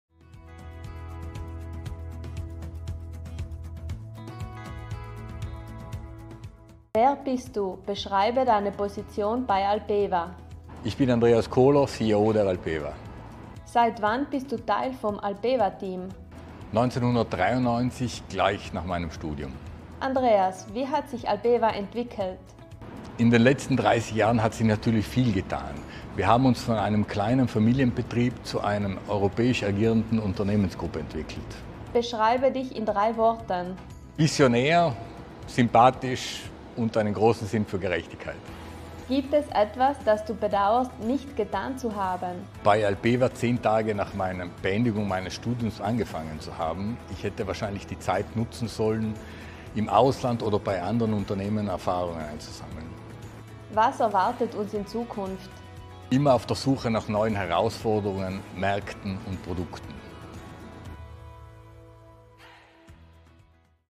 40 Jahre Alpewa: Ein exklusives Interview mit CEO Andreas Koler
Alpewa zelebriert den herausragenden Meilenstein seines 40-jährigen Jubiläums, ein Zeugnis seiner bemerkenswerten Entwicklung seit der Gründung. Zu diesem besonderen Anlass sind wir stolz darauf, die Einsichten und Reflexionen von Andreas Koler, dem CEO von Alpewa, mit euch zu teilen.